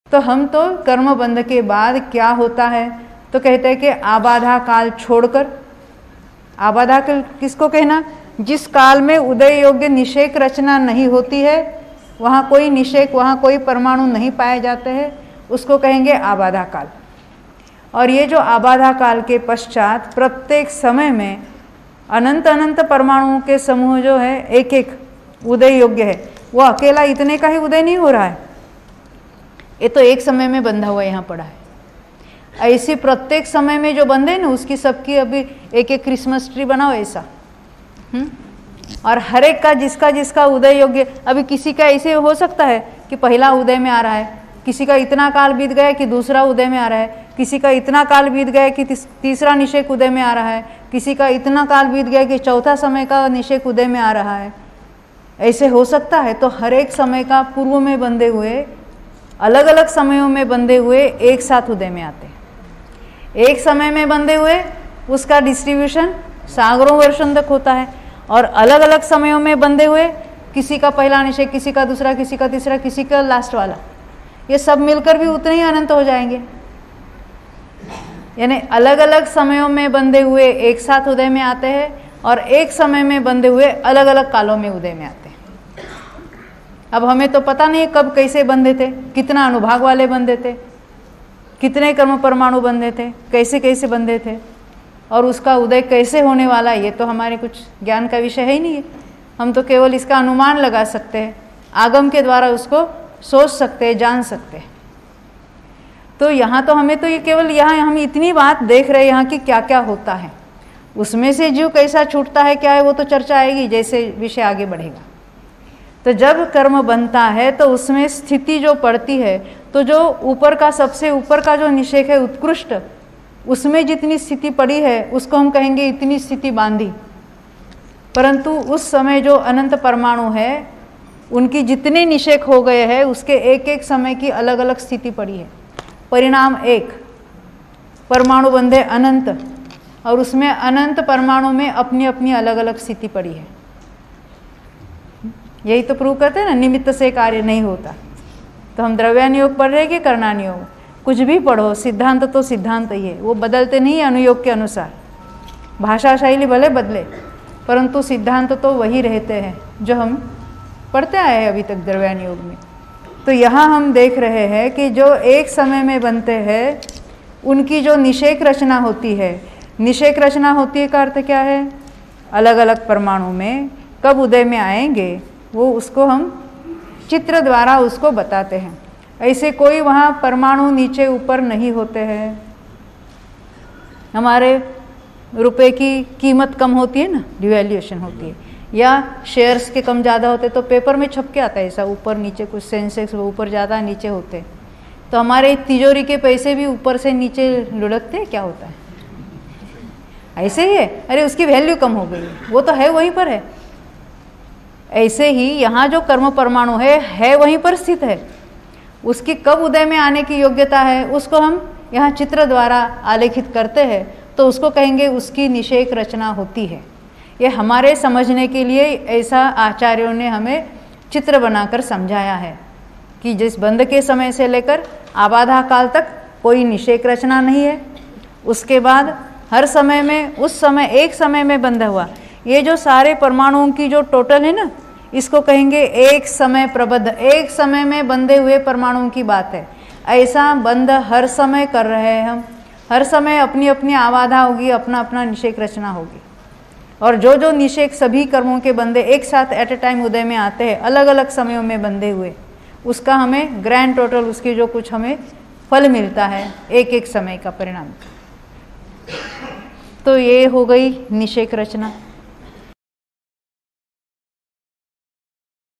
KK05-04 आबाधाकाल, निषेक रचना
५. निषेक रचना और स्पर्धक रचना

आचार्य श्री नेमिचन्द्र सिद्धान्तचक्रवर्ती ने गोम्मटसार कर्मकाण्ड ग्रंथ की रचना की है। उसीके आधार से पंडित टोडरमलजी ने ढूंढ़ारी भाषा में सम्यग्ज्ञानचन्द्रिका भाषाटीका लिखी है। उसीका हिंदी अनुवाद डॉ. श्रीमती उज्ज्वला शहा ने किया है जिसके आधार से सम्यग्ज्ञानचंद्रिका कर्मकाण्ड विषय पर देवलाली में दो शिबिर मार्च २००९ तथा जून २०१० में हुये थे।

करणानुयोग के स्वाध्याय की प्रेरणा के लिये इस शिबिर में से कुछ विषय की झलकियां प्रस्तुत है। सम्यग्ज्ञानचंद्रिका कर्मकाण्ड विषय पर शिबिर के सभी प्रवचन